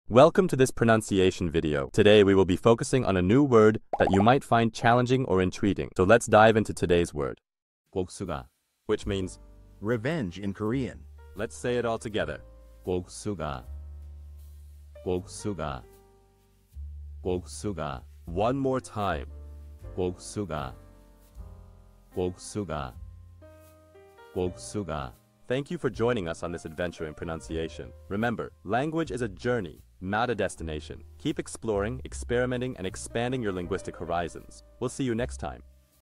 How to pronounce boksuga | 복수가 (Revenge in Korean)
Master the Pronunciation of 'boksuga | 복수가' - which means : Revenge in Korean 📝 with @PronunciationKorean 🎓🗣️ - [Your Guide to Korean]

Learn to pronounce 'boksuga' perfectly in Korean with our easy and engaging tutorial by @PronunciationKorean! 📖📽️ In this comprehensive video, we provide you with native speaker examples 🗣️👂 and phonetic breakdowns 🎵 to ensure you get the pronunciation just right.